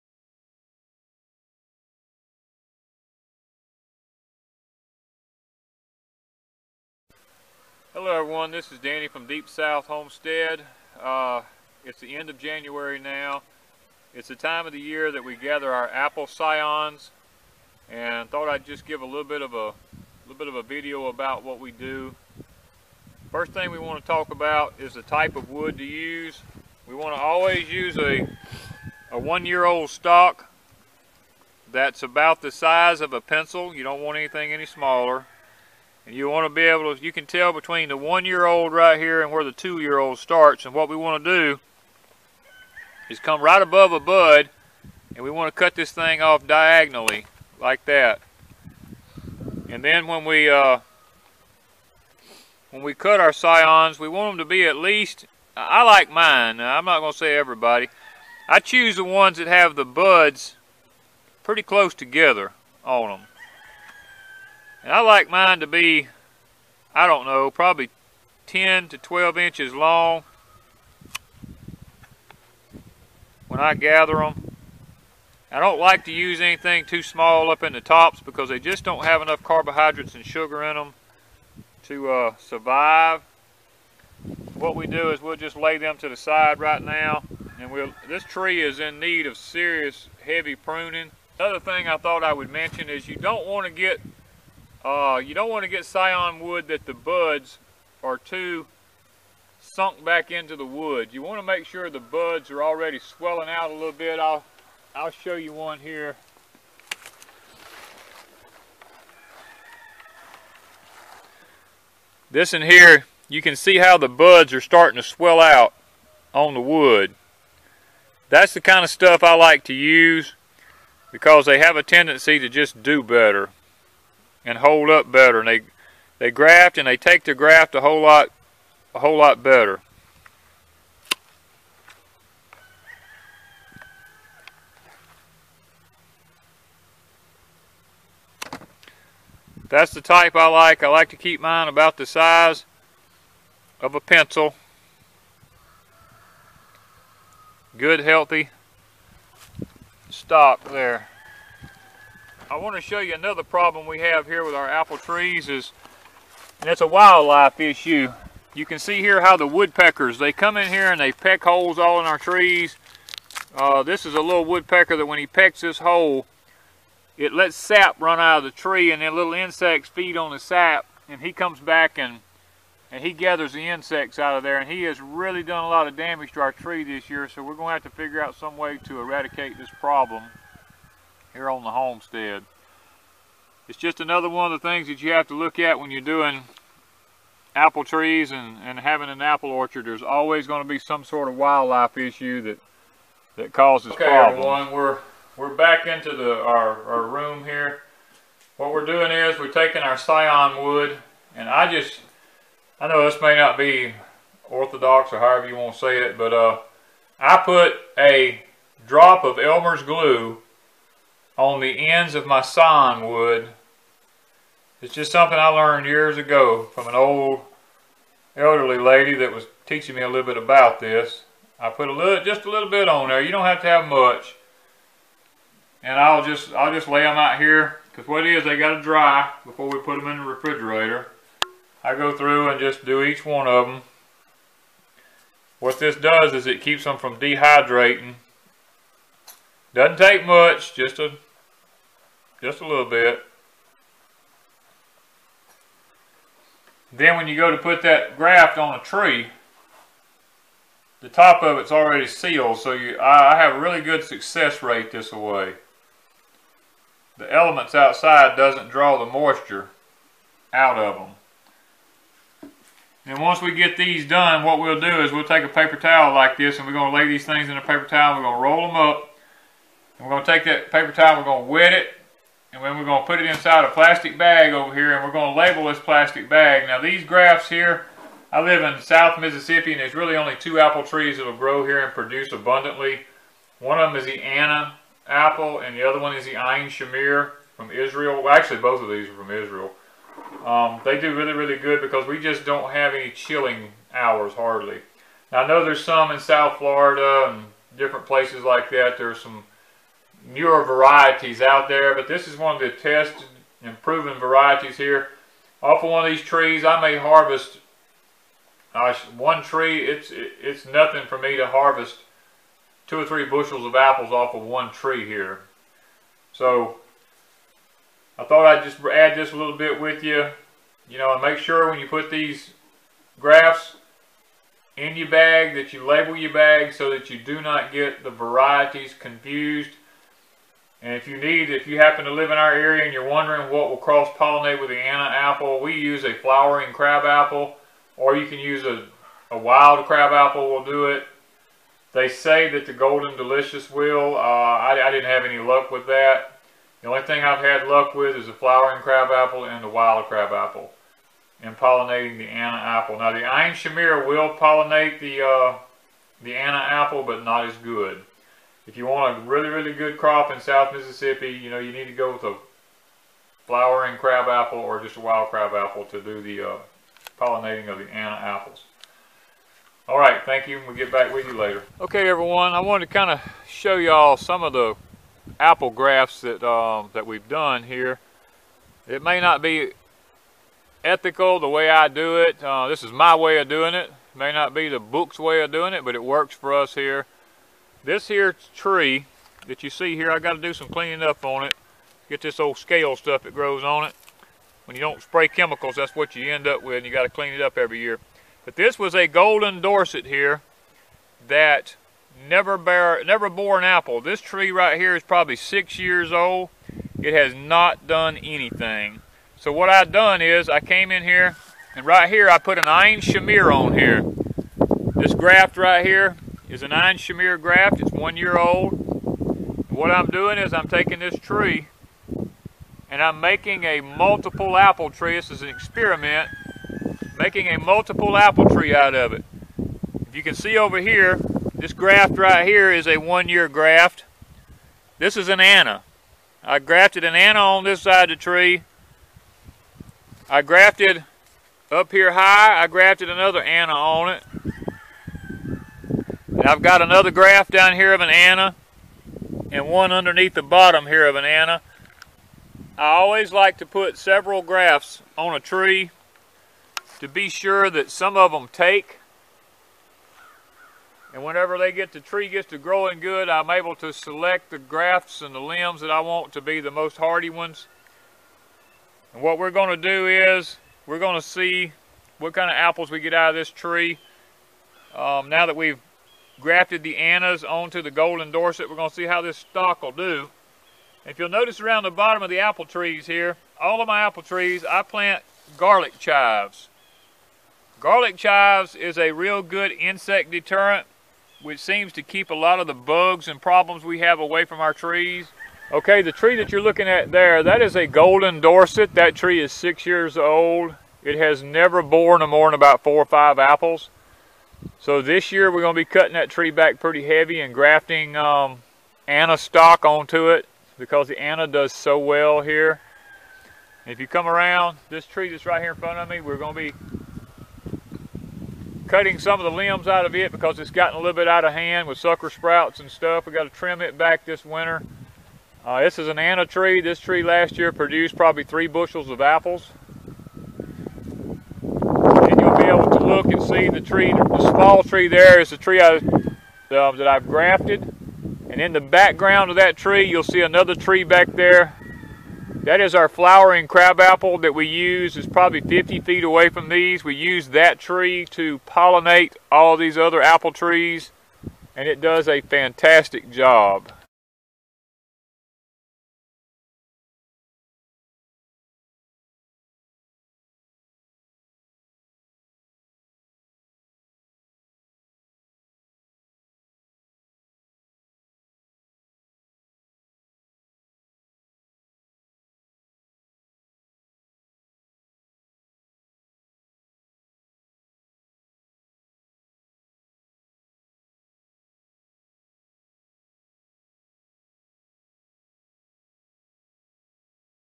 Grafting apples
How we graft apples and a few pics of the flowering crab apple, apples on the tree and some of the jelly and applesauce we canned.